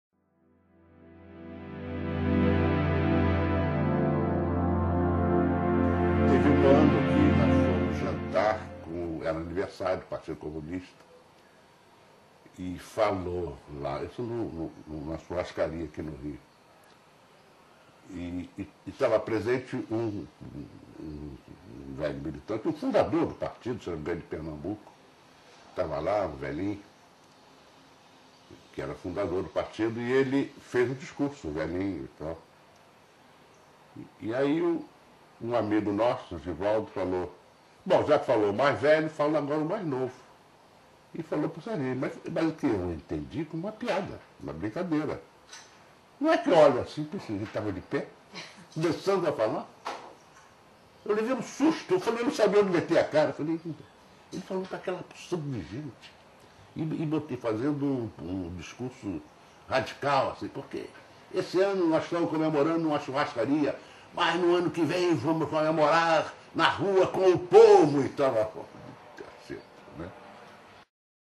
Sérgio Cabral - De Pai para Filho
Sérgio Cabral (Pai) conta sobre o dia em que o filho, ainda novo, discursou na frente de integrantes do Partido Comunista durante o jantar de comemoração do aniversário do partido.